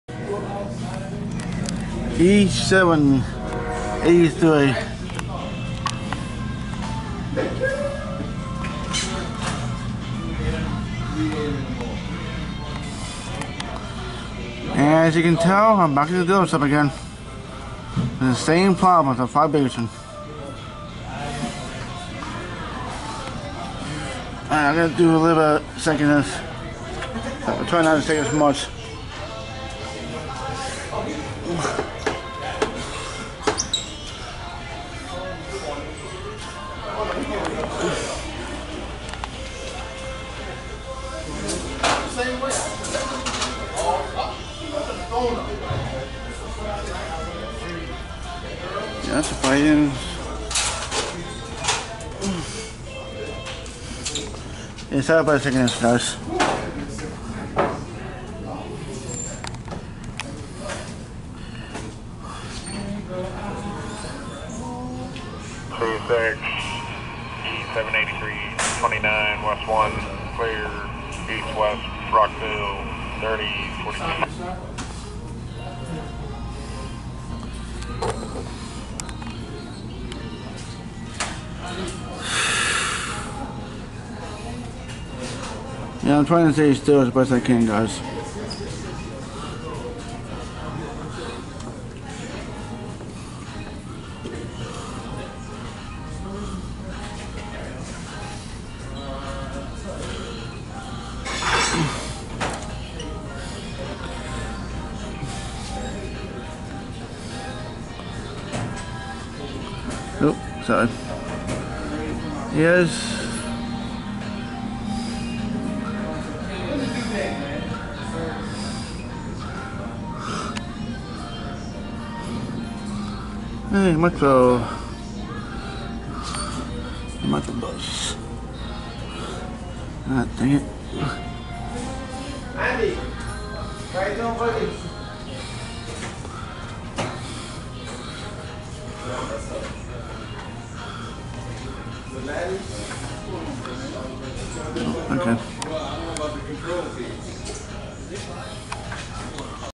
#7428 CSX E783-29 Empty Coal train
CSX E783-29 Empty Coal train is heading westbound on track 1.